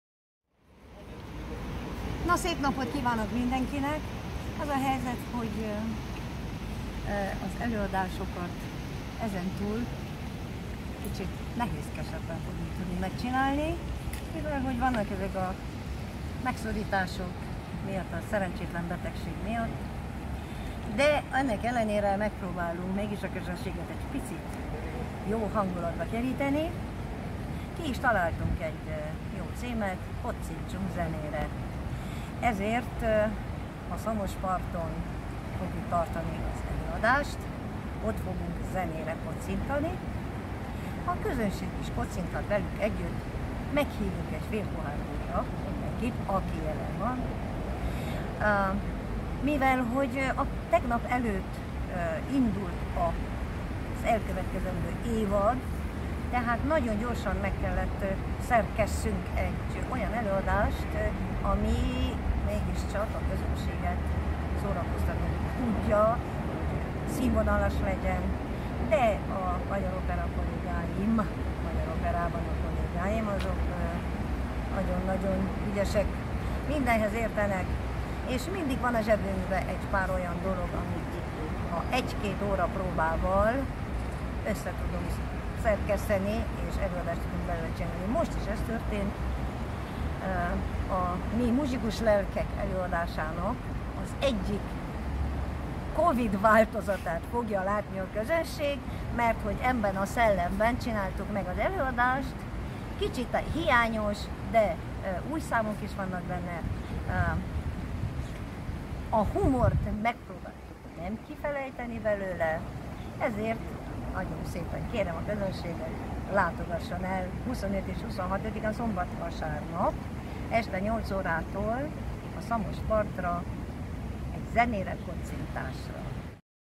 Incze G Katalin
HU:
Incze G. Katalinnak, a MI MUZSIKUS LELKEK... ZENÉRE KOCCINTUNK című előadás zenei vezetőjének beszámolója a hétvégi eseményekről!

🥂 MI, MUZSIKUS LELKEK... ZENÉRE KOCCINTUNK címmel rendhagyó előadásra várja közönségét július 25-én és 26-án este nyolc órától a Kolozsvári Magyar Opera társulata. Zenei vezető: Incze G. Katalin; koreográfus: Jakab Melinda; rendező: Gombár Annamária. Az eseményre a Magyar Opera udvarán kerül sor. 🎹🎺🥁🎻

Az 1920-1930-as évek átdolgozott slágereit Ádám János, Balázs Borbála, Fülöp-Gergely Tímea, Gergely Arnold, Jordán Éva, Kerekes Mária, Kis-Lukács Bernadett, Mányoki Mária, Nagy Szilvia Klára, Peti Tamás Ottó, Rétyi Zsombor, Rigmányi István, Szeibert István és Székely Zsejke tolmácsolásában láthatja-hallhatja a közönség.

Várjuk Önöket! ✨

RO:
Interviu cu Incze G. Katalin, conducătorul muzical al concertului SUFLET DE MUZICIAN despre spectacol!
🥂 Spectacolul SUFLET DE MUZICIAN în curtea Operei Maghiare în data de 25 şi 26 iulie de la ora 20❗🎻🥁🎺🎹

Soliști: Ádám János, Balázs Borbála, Fülöp-Gergely Tímea, Gergely Arnold, Jordán Éva, Kerekes Mária, Kis-Lukács Bernadett, Mányoki Mária, Nagy Szilvia Klára, Peti Tamás Ottó, Rétyi Zsombor, Rigmányi István, Szeibert István, Székely Zsejke

Conducător muzical: Incze G. Katalin
Coregrafie: Jakab Melinda
Regia: Gombár Annamária
Locația:  Curtea Operei Maghiare

Vă aşteptăm cu drag! ✨